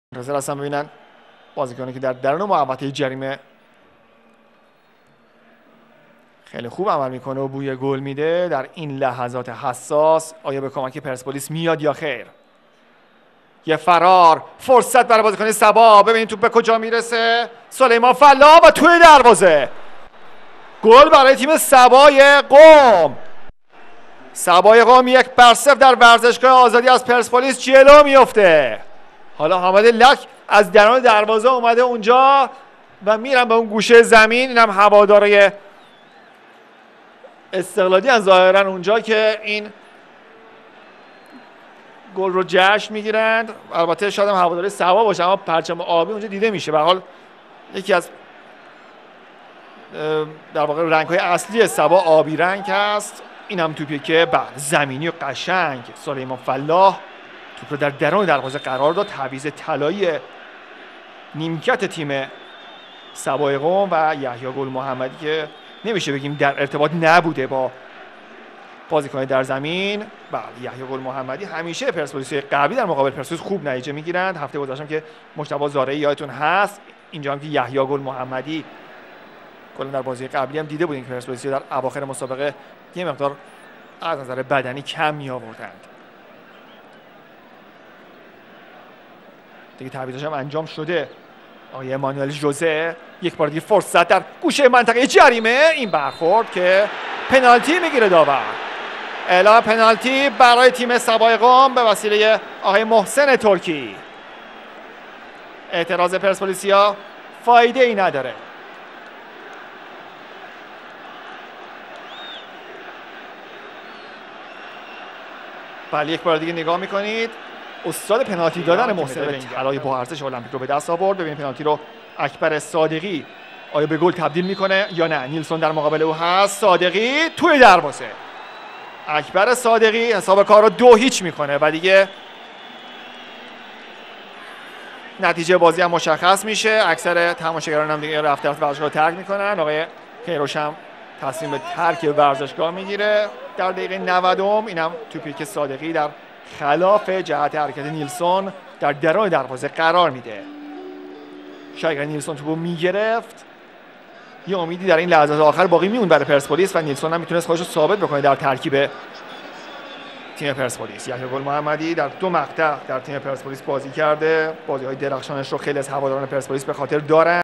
[IPL 12/13] Perspolis Tehran - Saba Battery (Match Highlights)
IPL 12/13 | Perspolis Tehran 0 - 2 Saba Battery
75' [0-1] Milad Soleiman Fallah
90' [0-2] Akbar Sadeghi (P.K.)